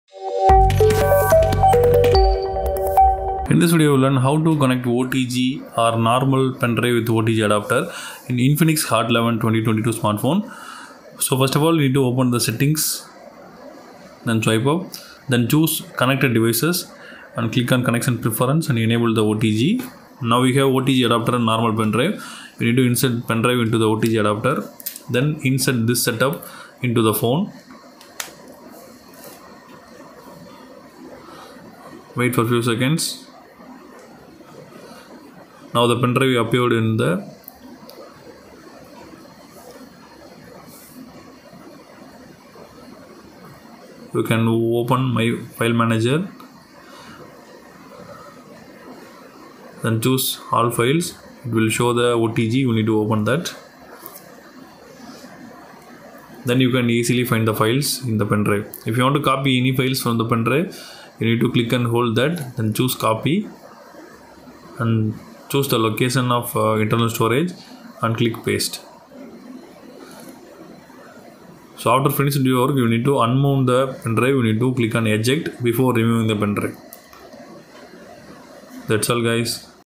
How to OTG in Infinix Hot 11 2022
Hey Friends  ! Checkout this new video of How to OTG in Infinix Hot 11 2022